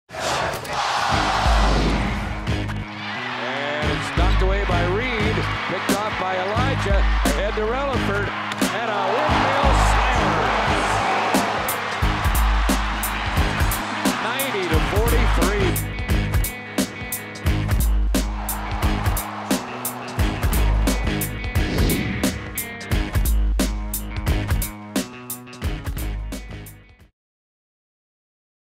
Travis Releford - Kansas (Dunk of the Year Finalist)
6'5" guard Travis Releford of Kansas throws down a nasty windmill dunk against UMKC. To vote for Releford's dunk as the 2011 Dunk of the Year, go